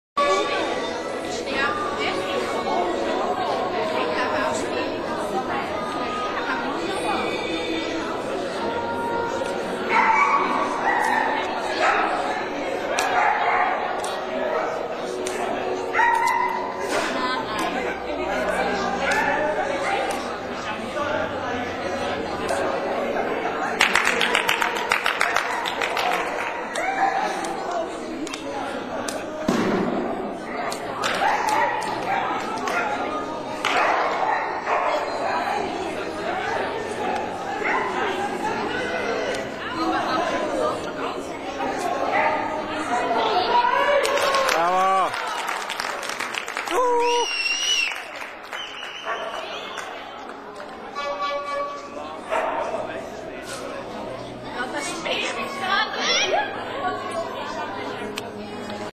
Sole Agility 3 Biasca Gennaio 2008
Sole e Danilo doing Agility 3 in Biasca January Meeting, they got 4th place